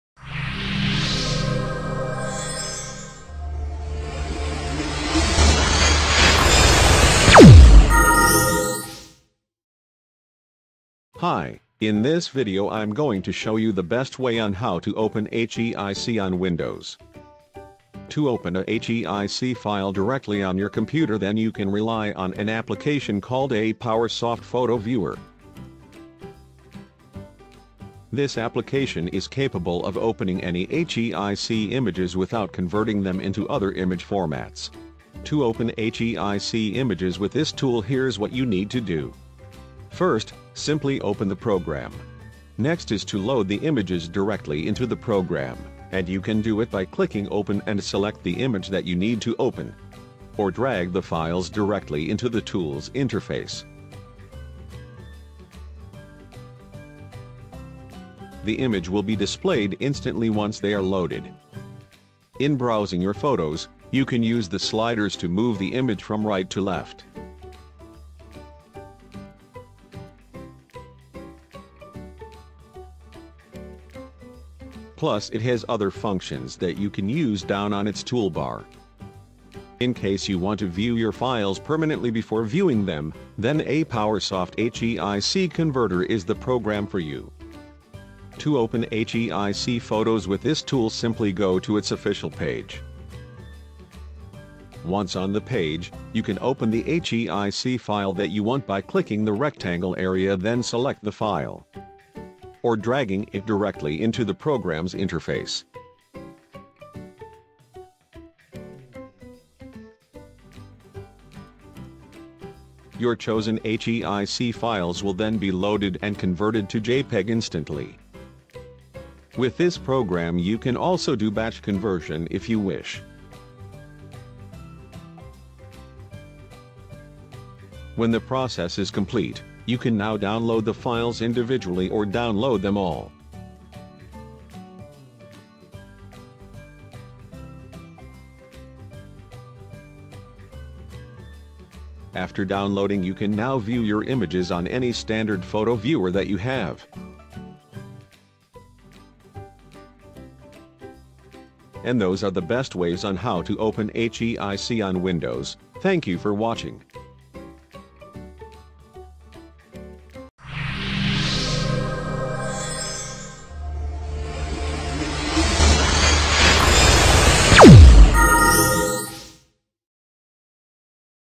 How to Open HEIC on Windows PC for Free
HEIC is a new image format adopted by Apple in its new iOS 11, and it greatly helps to save iPhone storage. This video shows how to open HEIC images on Windows PC by using Apowersoft Photo Viewer, an image viewing program that can be used to view many types of images. Also, it introduces a practical way to convert HEIC to JPG. Watch this video to learn more.